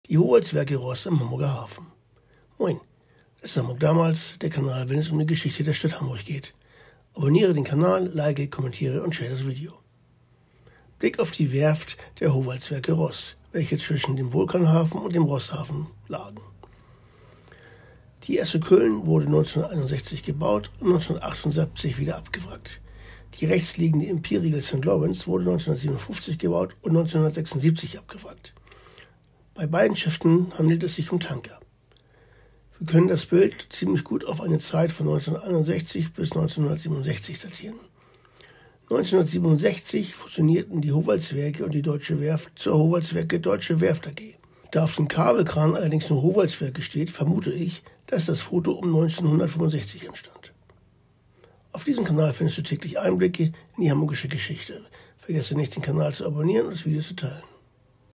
HAMBURG damals - Hafen um 1965
Die Howaldtswerke Ross im Hamburger Hafen.

Quelle: Sammlung Hamburg damals